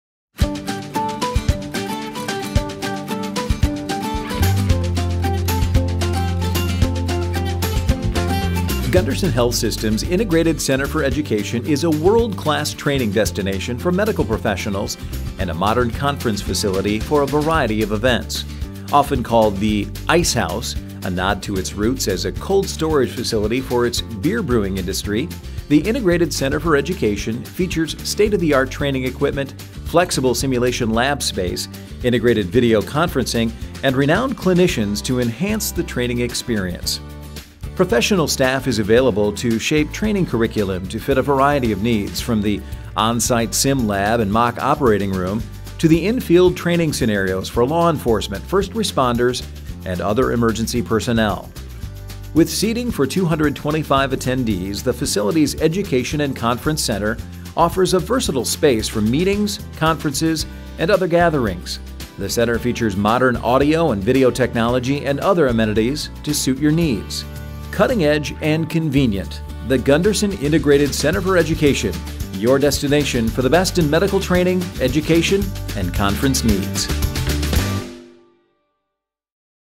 The Integrated Center for Education at Gundersen Health System
Gundersen Health System’s Integrated Center for Education is a world-class training destination for medical professionals and a modern conference facility for a variety of events.

Often called the ‘ICE House,’ a nod to its roots as a cold storage facility for the beer brewing industry, the Integrated Center for Education features state-of-the-art training equipment, flexible simulation lab space, integrated video conferencing and renowned clinicians to enhance the training experience.

Professional staff is available to shape training curriculum to fit a variety of needs, from the on-site sim lab and mock operating room, to in-the-field training scenarios for law enforcement, first responders and other emergency personnel.

With seating for 225 attendees, the facility’s Education and Conference Center offers a versatile space for meetings, conferences and other gatherings. The center features modern audio and video technology and other amenities to suit your needs.

The Gundersen Integrated Center for Education:
Your destination for the best in medical training, education and conference needs.